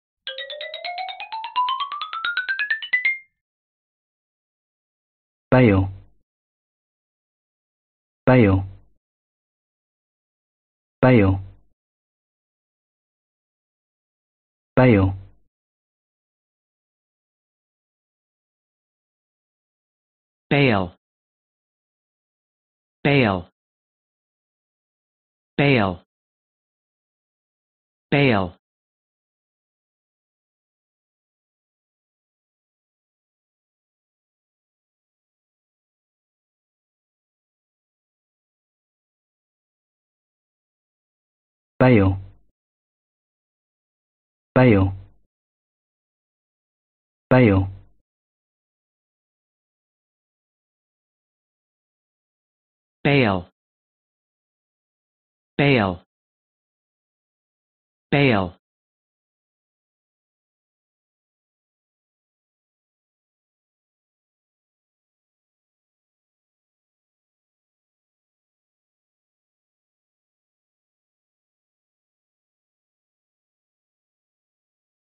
英単語 bail sb/sth out 発音と読み方
英単語bail sb/sth outの発音動画です。

イギリス英語での発音記号は「beɪl」でアメリカ英語での発音記号は「beɪl」です。